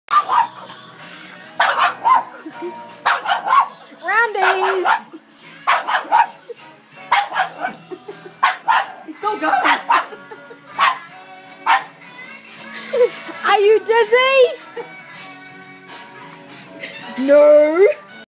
Spinning Pug Dog
Chuey loves doing "roundies" .